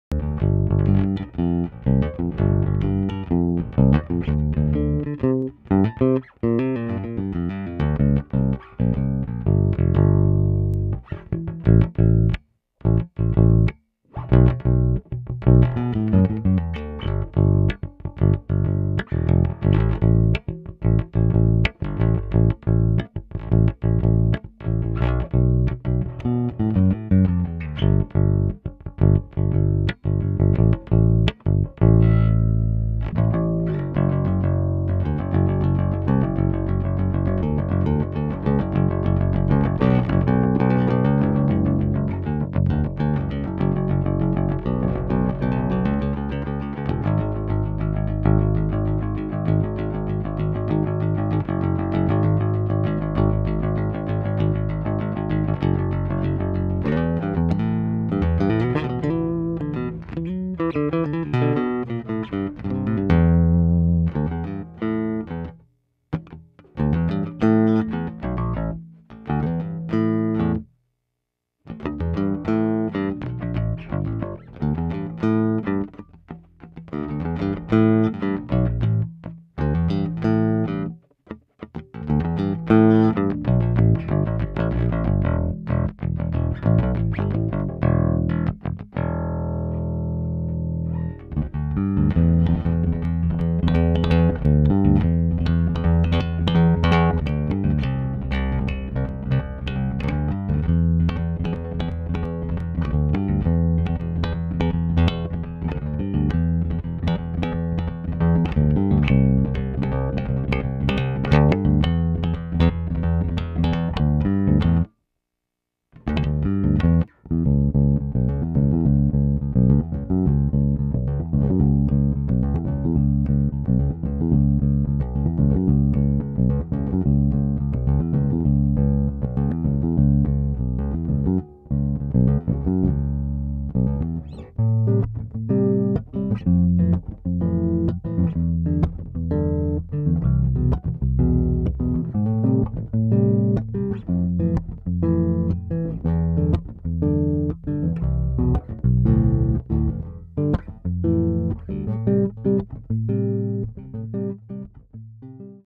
Porter P-Bass Pickup Demo
Demo of our P-bass pickup Set, in a peavy bass.